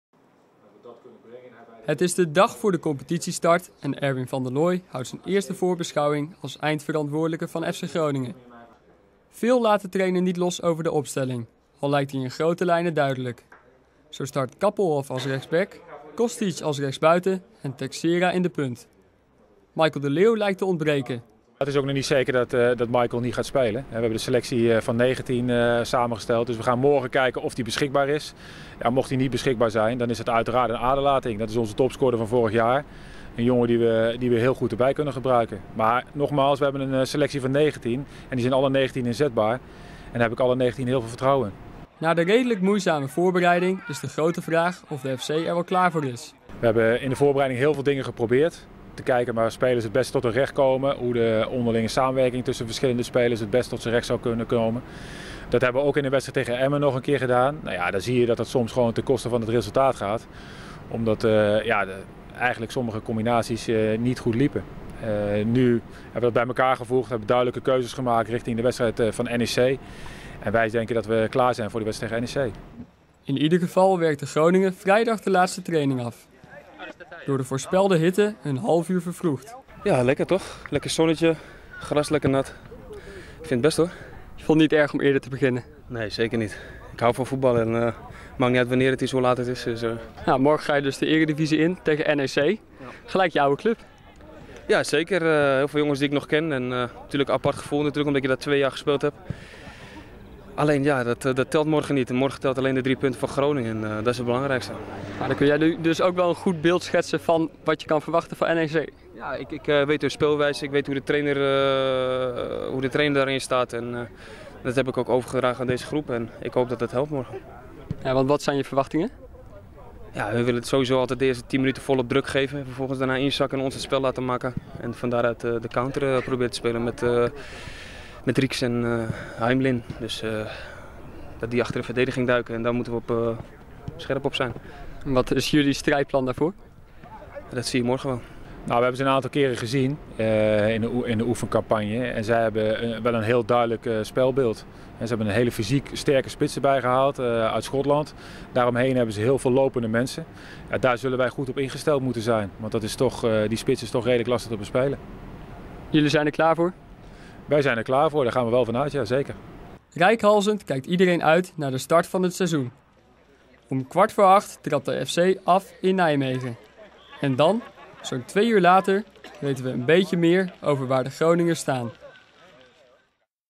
Texeira vervangt De Leeuw in de spits
Spits Michael de Leeuw ontbreekt zaterdagavond in het eerste competitieduel van FC Groningen tegen NEC, vanwege een blessure.De Leeuw liep woensdag tijdens de training een blessure op aan het bovenbeen. Hij is daar onvoldoende van hersteld. David Texeira vervangt hem in de spits.FC Groningen begint zaterdagavond in Nijmegen waarschijnlijk in deze opstelling: Bizot; Kappelhof, Letschert, Botteghin, Wijnaldum; Van der Velden, Lindgren, Chery; Kirm, Texeira, Kostic.